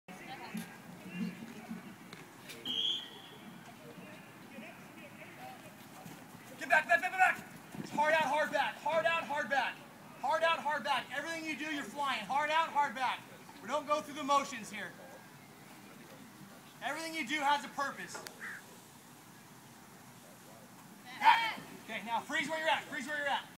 Base Running Drills 9/12 (Part 2)
It's a beautiful day for baseball, as PURE Baseball's fall program continues with a series of base running drills from Sept. 12